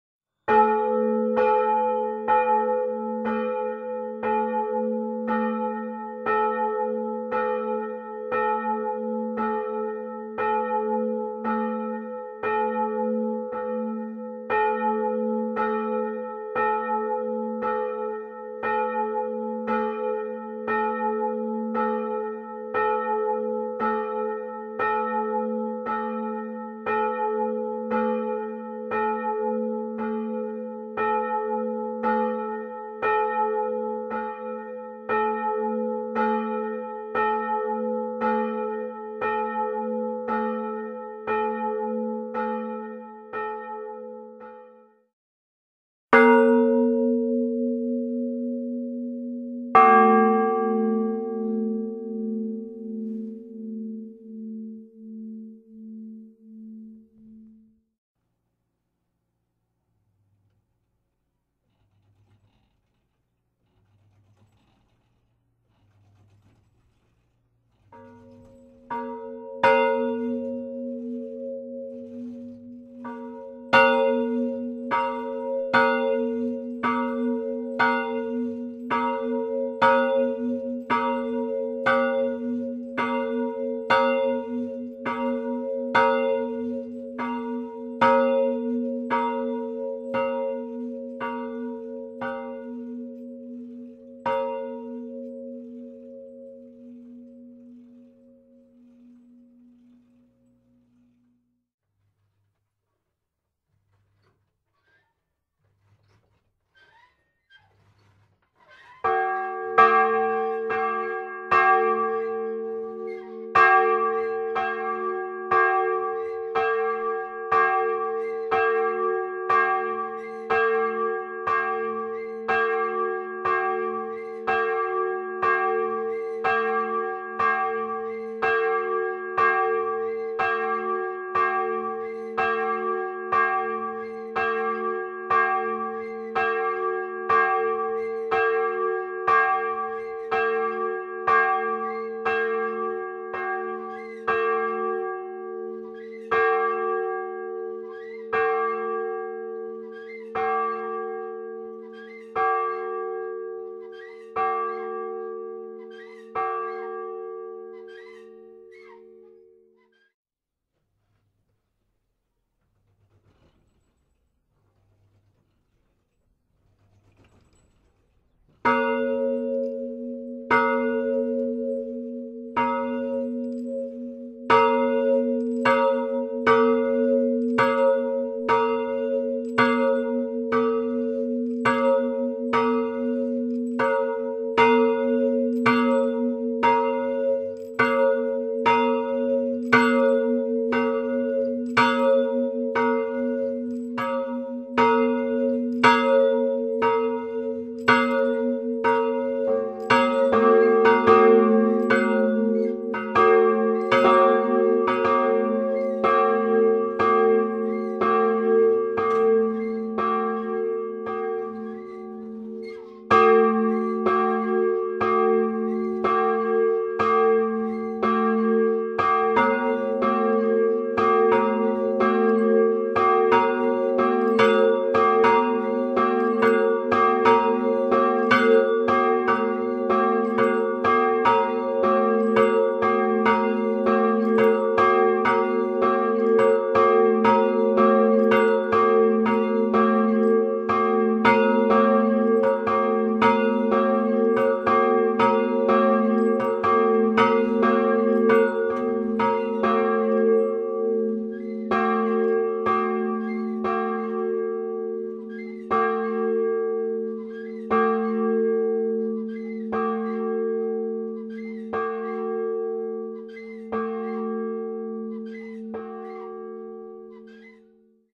Zvonovi podružnične cerkve Svetega Urha na Cerovcu pri Trebelnem
Cerkvica spada v župnijo Trebelno.
V zvoniku sta dva zgodnja primera zvonov iz Jeseniške livarne KID. Kaj več o starih zvonovih ni znanega.
Zvonovi:
1. KIG Jesenice - 1918 - g'-4/16
2. KIG Jesenice - 1918 - b'+6/16